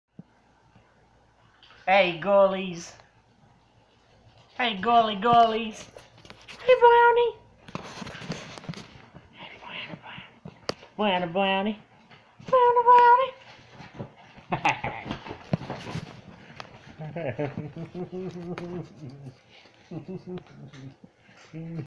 Getting Assaulted by Brownie the Dachshund..😂🤣🐕
Short and sweet video of me getting attacked by the most vicious of animals lol